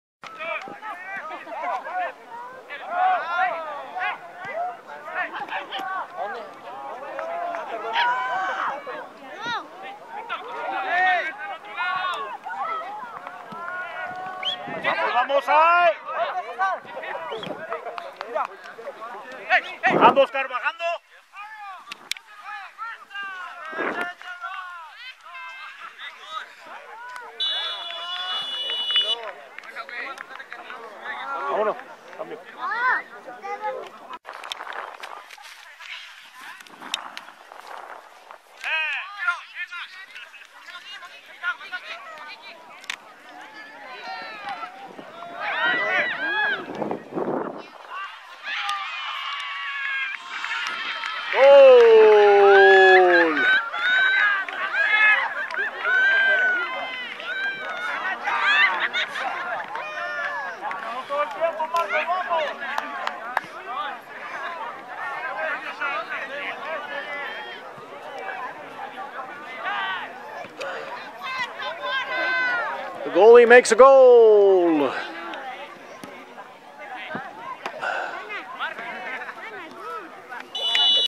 04-16-13 U-17 Elite Soccer: Goal Highlights v Real Madrid Lomeli
Video of the last 2 Elite goals: Raul Ramirez & Josh Miller. U-17 Elite shut out Real Madrid Lomeli 4-0.